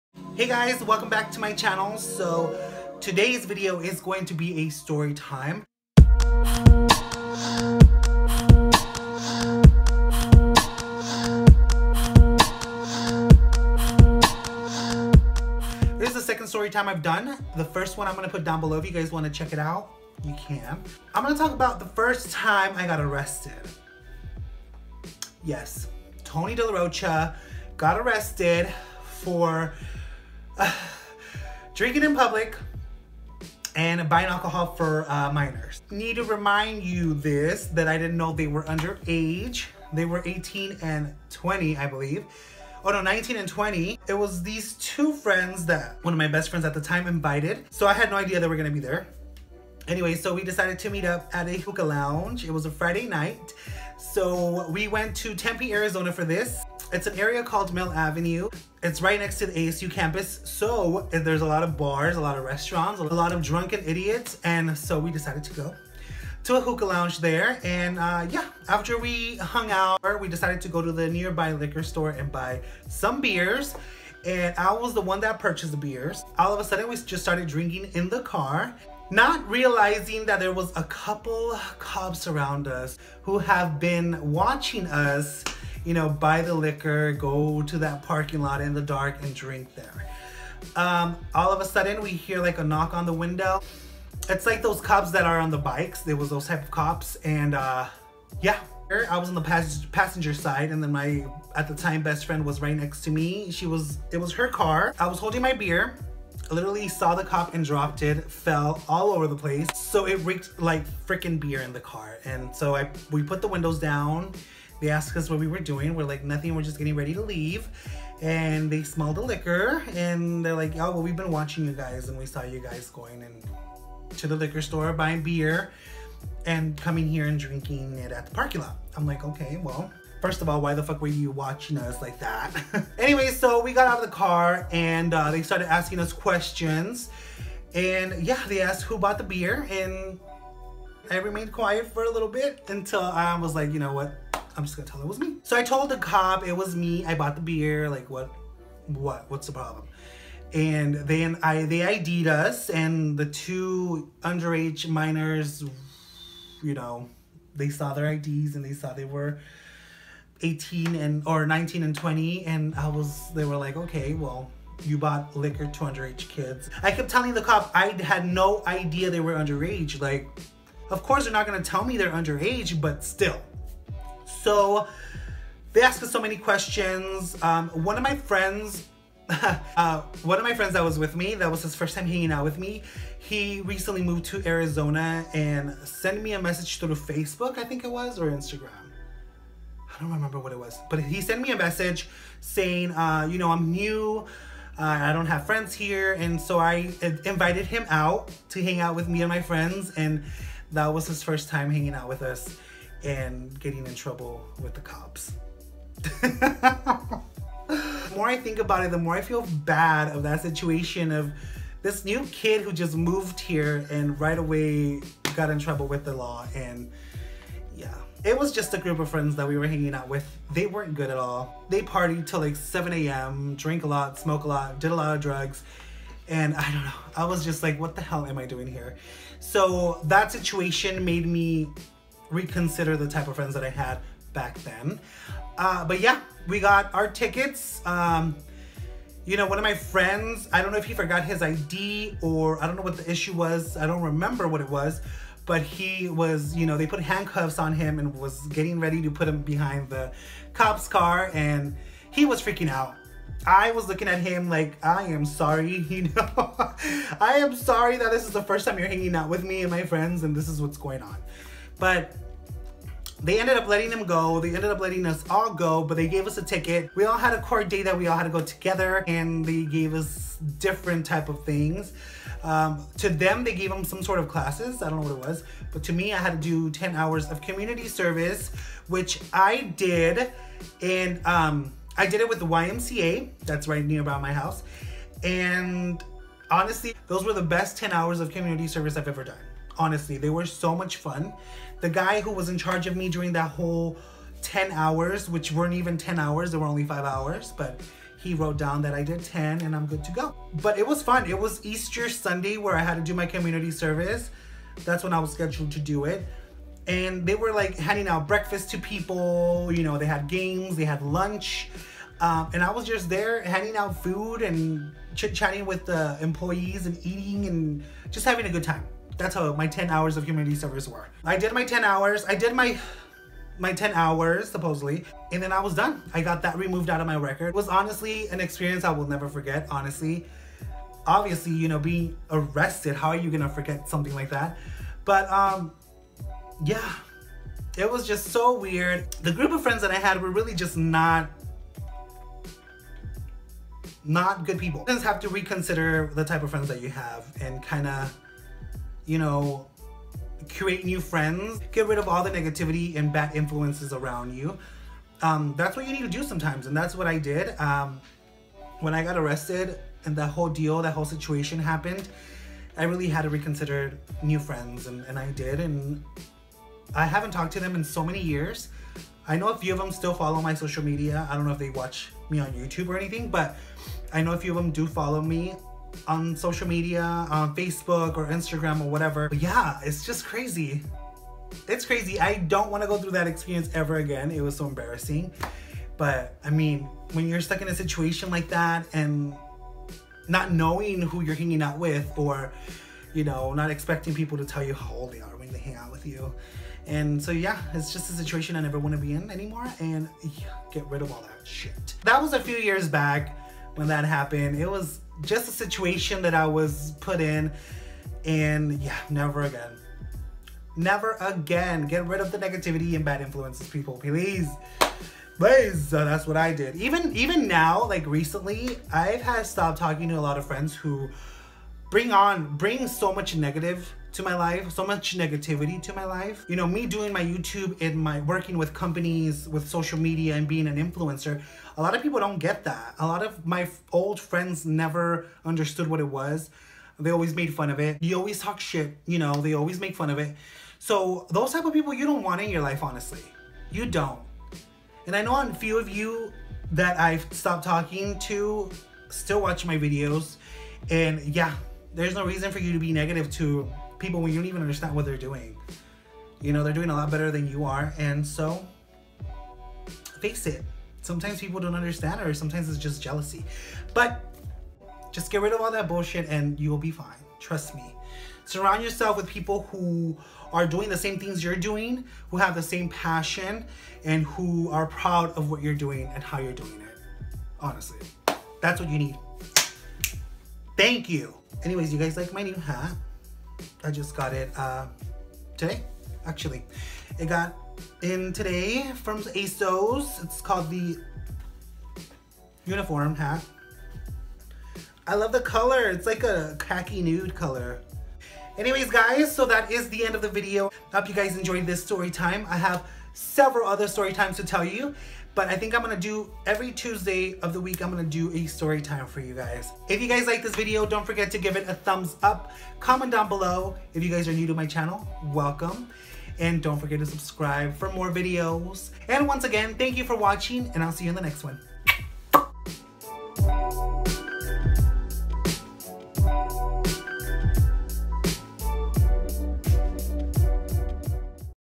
I Was Arrested (Story Time) || TONYDELAROCHA
Hey guys! Welcome to my second story time 💋